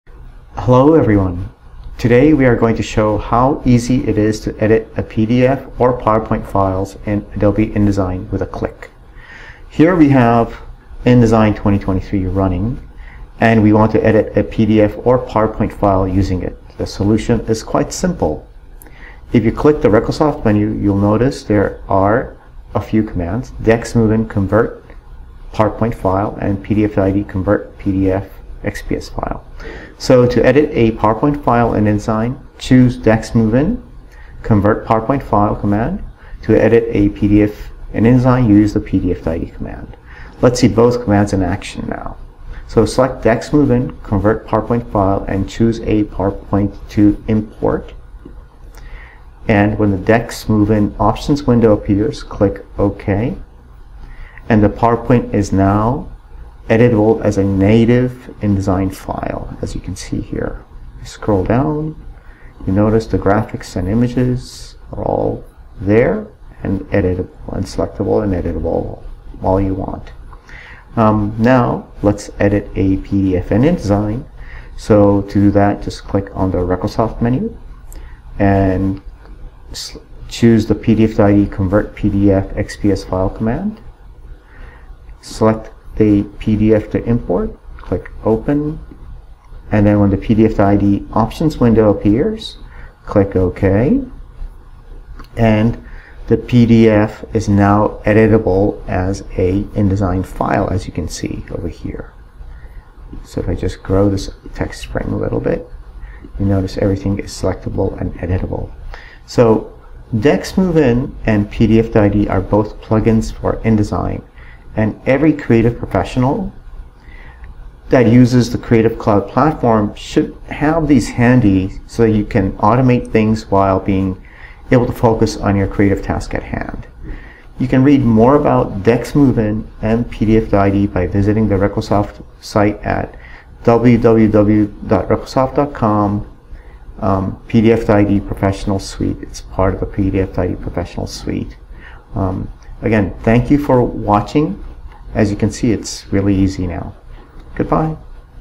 Edit PowerPoint & PDF files in InDesign in 1-click | PDF2ID Suite 2023
Editing a PowerPoint file or PDF file is now possible with 1-click.

1. Click Recosoft menu and choose DecksMoveIn and import a PowerPoint into InDesign to edit.
2. Click Recosoft menu and choose PDF2ID and import a PDF file into Adobe InDesign to edit.

PDF2ID Professional Suite 2023 now makes it possible for anyone who uses the Adobe Creative Cloud platform to easily edit PowerPoint and PDF files as native InDesign files.

You can also edit FIGMA,, Google Slides and Keynote in InDesign usingDecksMoveIn for InDesign.